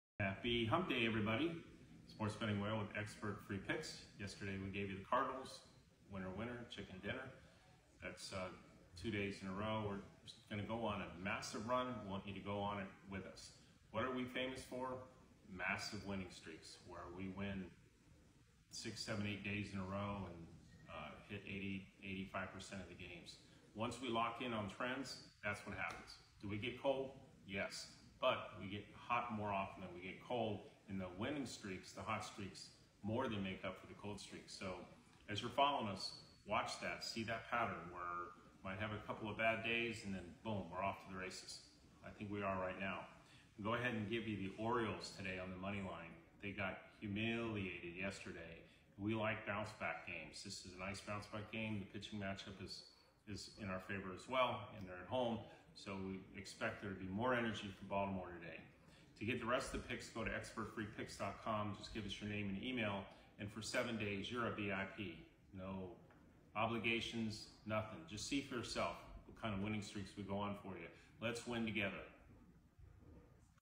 Sports Betting Whale Picks for Wednesday, September 18, 2024
is the first ever 100% FREE sports handicapping service.

Yesterday we went 2-0 on the VIP Selections and 1-3 on Regular Selections creating a slight overall profit for the day.

Let's go on a huge run together 🙌baseball